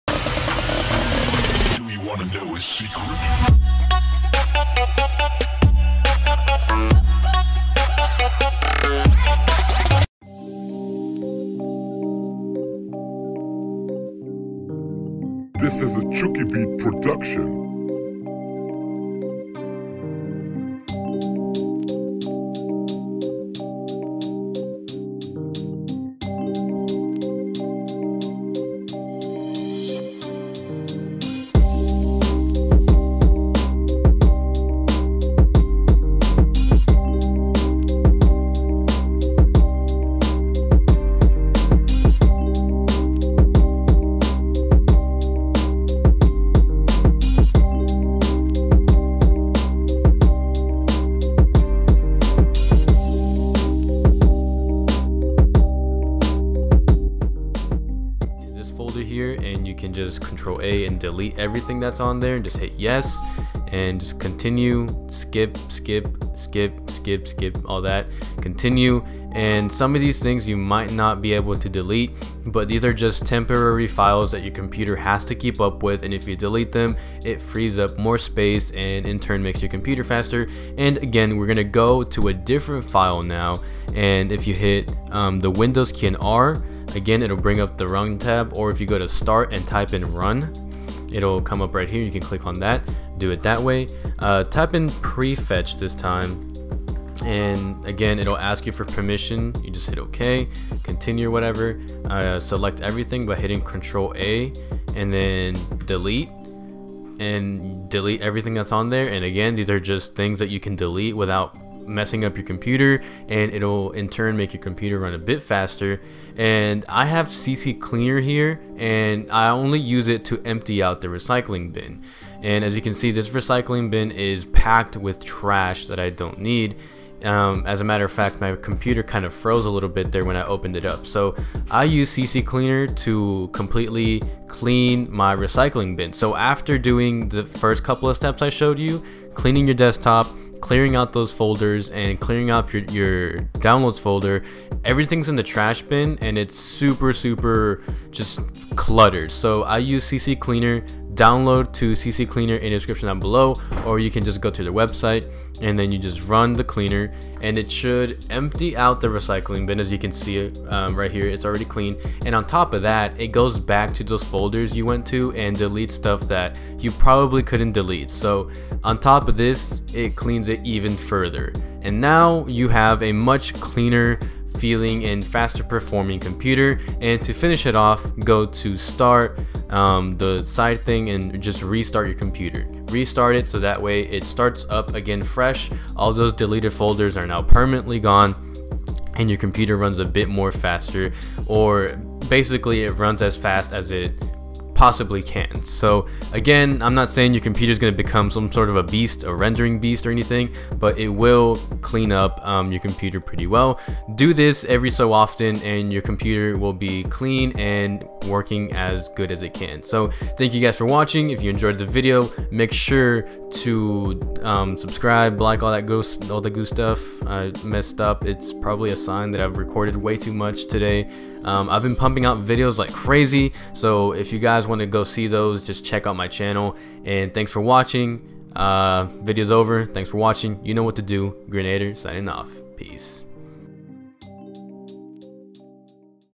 5 Ways You Can Boost Your PC's Performance
Is your PC slow? Well today I will show you 5 Ways You Can Boost Your PC's Performance!